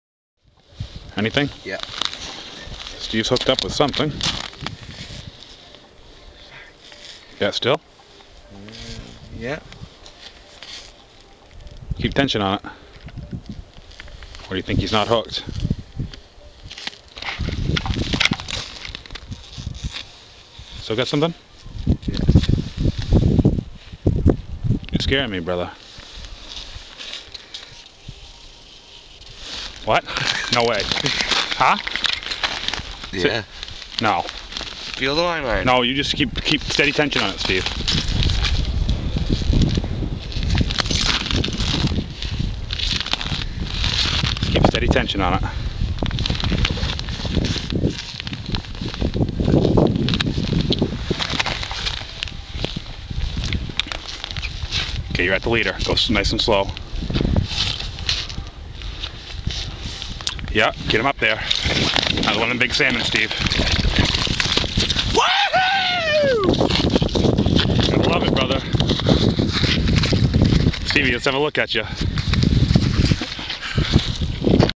Maine Salmon caught February 16 2008
This fish weighed 4 pounds and measured 22.25 inches in length. Caught at No Name Pond on Hush Hush Road. Haha